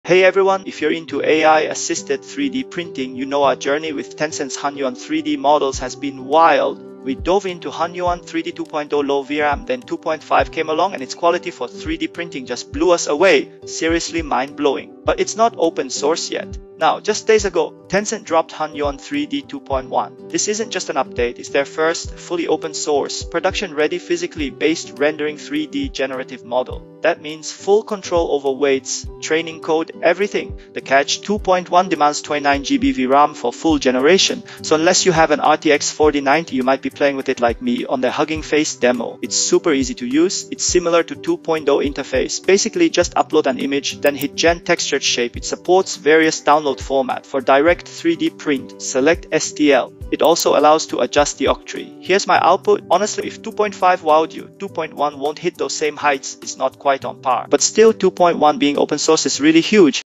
Hunyuan3D 2.1 RELEASED: Better Than 2.0, But What About 2.5? Free
Tencent Just Released Hunyuan3D 2.1 OPEN SOURCE! (The TRUTH vs. 2.5 for 3D Printing)
Get ready Tencent just dropped a bombshell: Hunyuan3D 2.1 is now open source! We've been on an incredible journey with these AI models for 3D printing, from the VRAM-friendly Hunyuan3D 2.0 to the absolutely mind-blowing quality of Hunyuan3D 2.5.
We're diving into the brand-new Hunyuan3D 2.1. This is Tencent's first "fully open-source, production-ready Physically Based Rendering 3D generative model," meaning they've released everything – model weights, training code, and more! We'll show you how it stacks up against 2.0 and the incredible 2.5.
While 2.1 brings clear improvements over 2.0, offering better fidelity for your local 3D model generation, be warned: it's a VRAM beast, demanding a whopping 29GB for full generation! We'll also take a quick look at my own outputs generated directly from their Hugging Face demo.
Is Hunyuan3D 2.1 the next big leap for your AI-powered 3D prints? Watch to find out if this demanding, but fully open-source, model is the right tool for your high-end setup.

- Hunyuan3D 2.0 video: 🎬 - AI-Powered 3D Printing - From Idea to 2D to 3D Model with Hunyuan3D-2-lowVRAM for FREE! + Bonus tip for Blender!